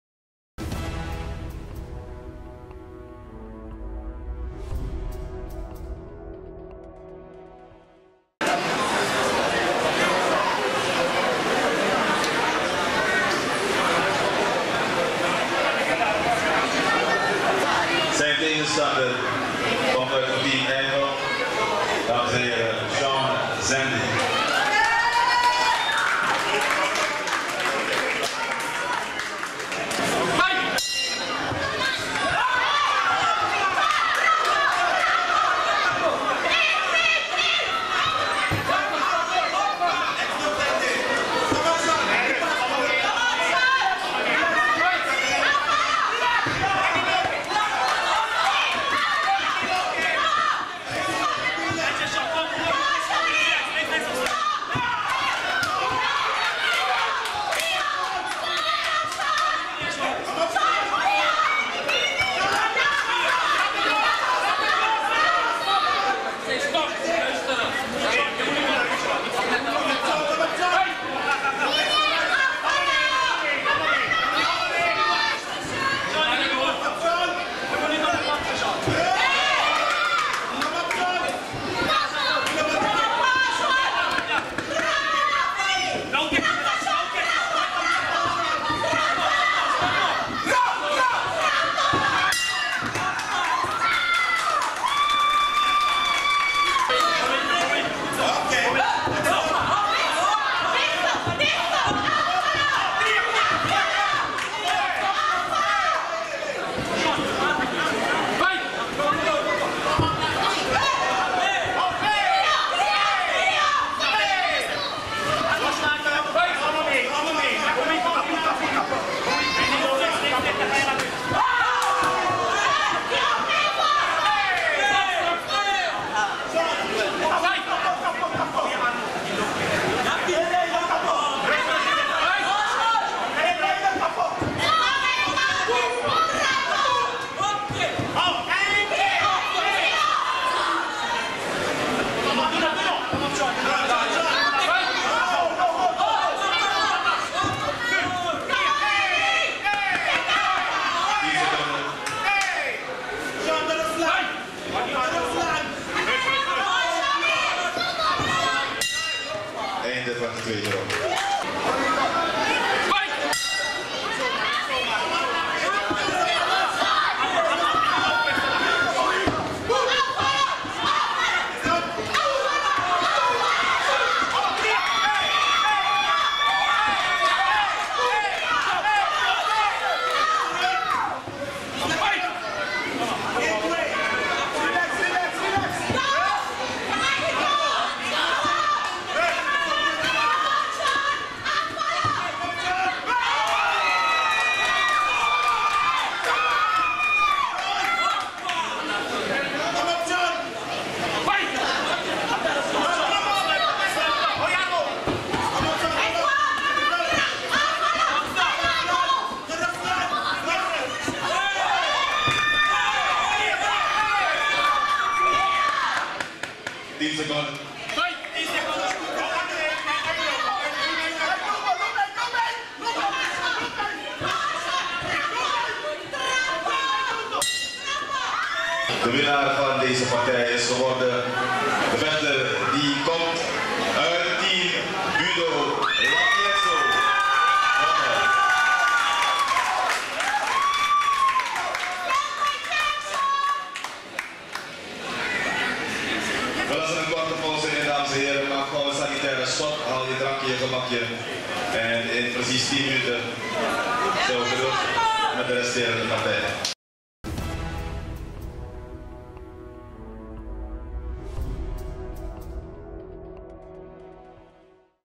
Clubwedstrijden Team Yvel. Seann Sandy vs Lafyenso Bommel
Team Wisse Presents: Sean Sandy vs Lafyenso Bommel